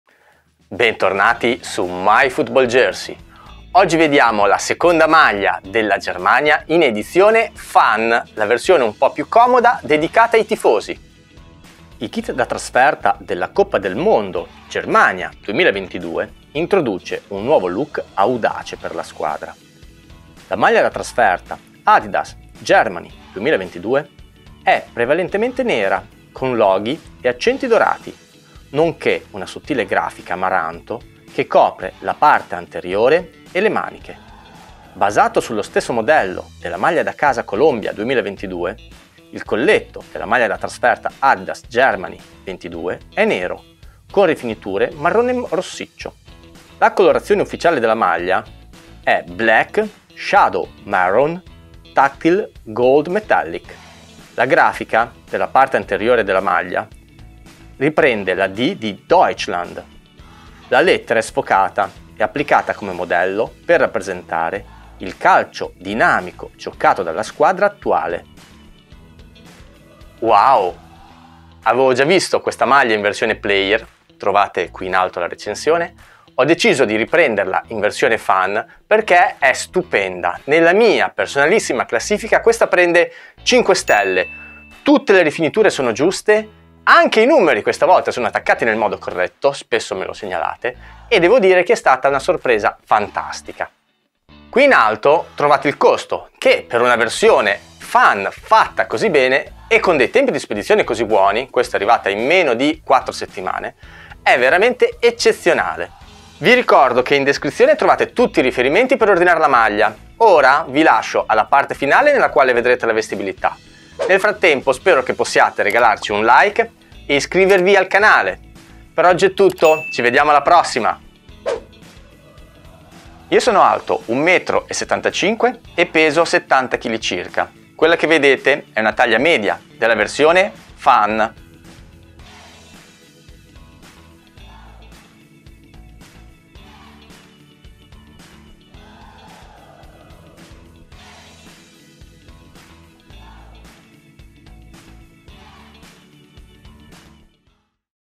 Germany Away Jersey Qatar 2022 Kimmich (JeoFC) Fan Version Unboxing Review
Discover the Germany Away Jersey Qatar 2022 (Fan / Stadium / Replica Version)
It is customized with Kimmich number #6! Watch till the end to see how the Germany Away Jersey Qatar 2022 fits!

🕘 Timecodes
00:00 Overview
00:21 Details View
01:10 Ranking
02:01 Dimensions & Try On

Please watch the Germany Away Jersey Qatar 2022 video review in 1080p (HD) 📺

Follow me also on TikTok @my.football.jersey to watch the Germany Away Jersey Qatar 2022 video review in a different way. 💥